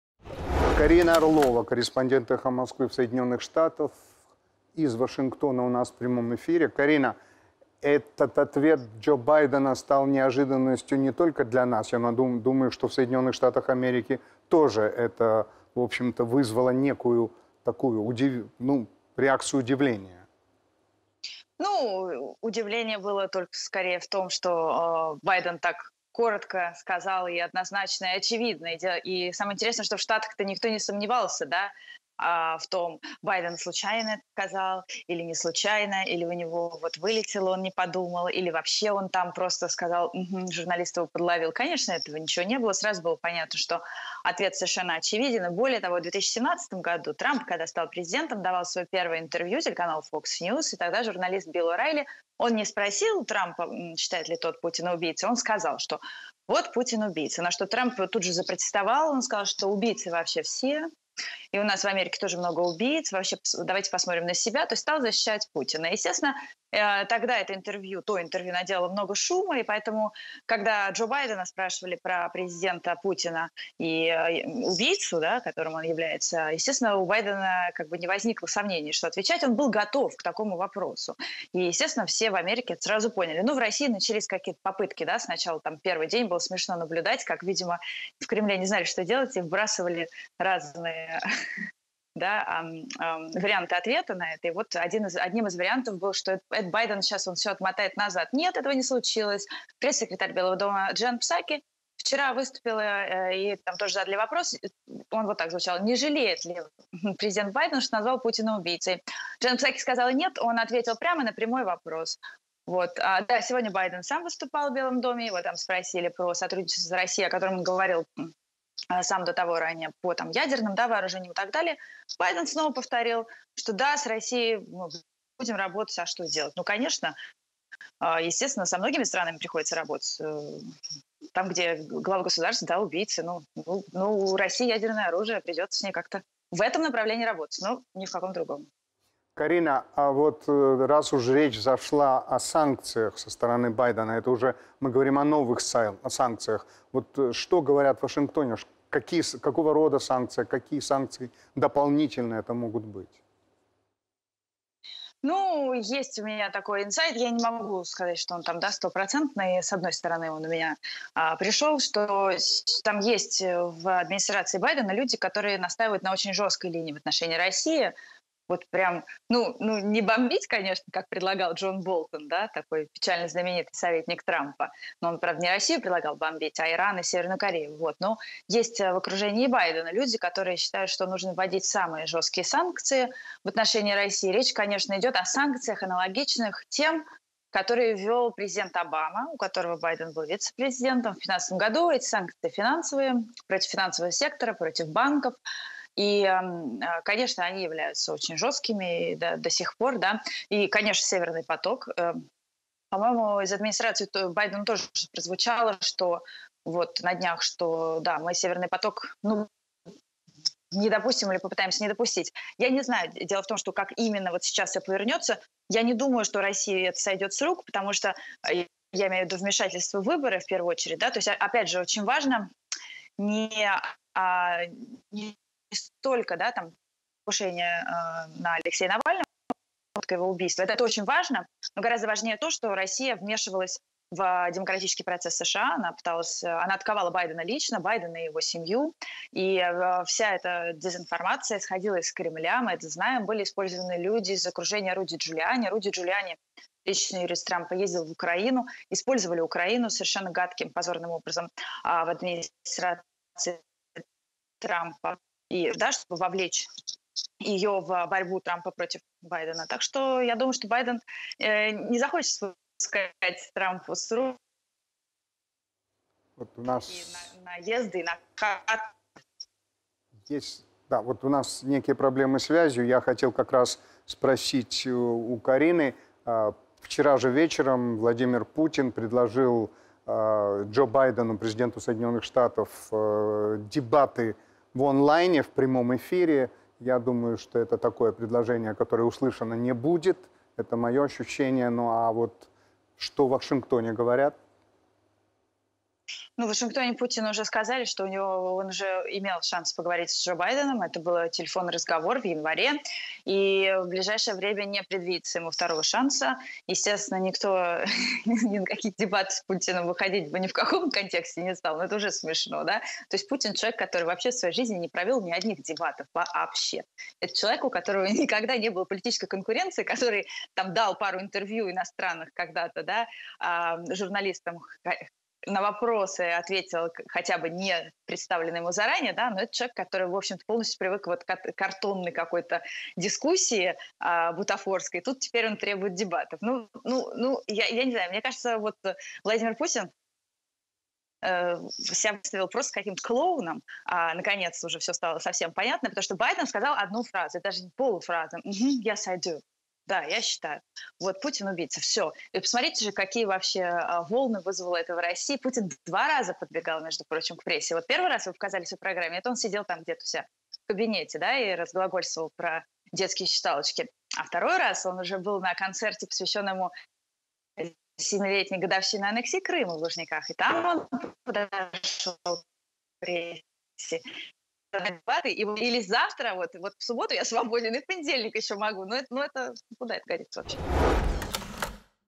«В Кремле, видимо, не знали что делать», – Карина Орлова про слова Джо Байдена о Владимире Путине
Корреспондент «Эхо Москвы» в США Карина Орлова прокомментировала, как в Вашингтоне и Москве отреагировали на слова Джо Байдена о Владимире Путине и на реакцию президента РФ.

Они говорят, а решает народ.